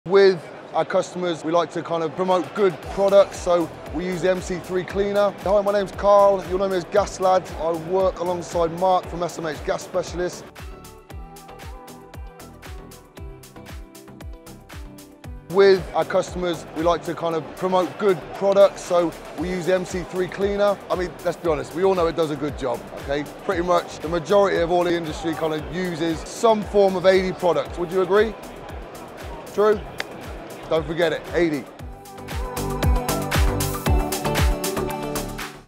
ADEY | Review & Testimonial | MagnaClean® | MC3+® Cleaner | Gasladd
Do you use ADEY water treatment solutions?

Carl Ladd, also known as @gasladd on Instagram, explains why he uses ADEY ProCheck® and MC3+® with all his customers.

Which ADEY products do you use with your customers?